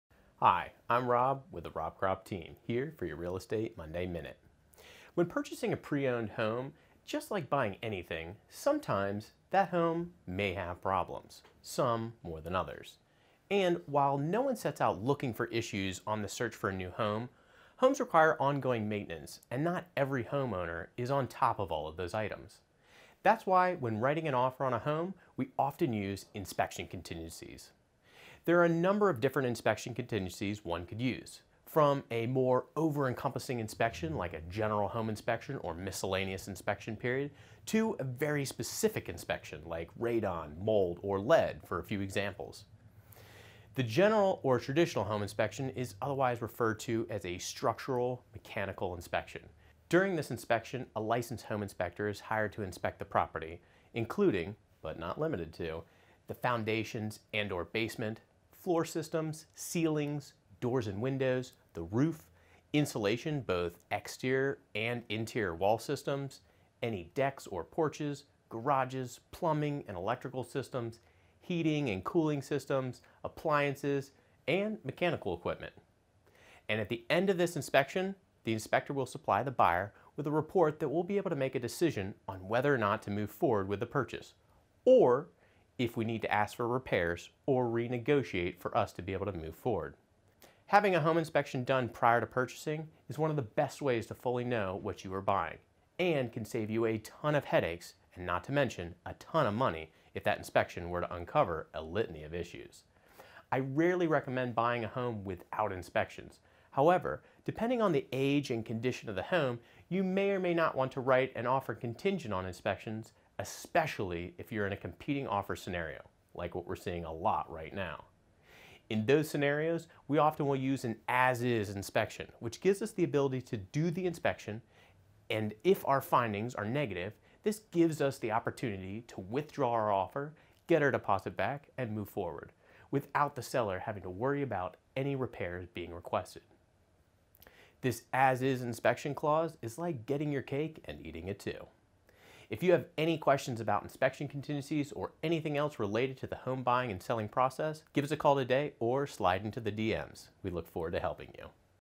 Monday Minute - Home Inspection Contingency
What is an inspection contingency? Why does it matter when buying or selling your home?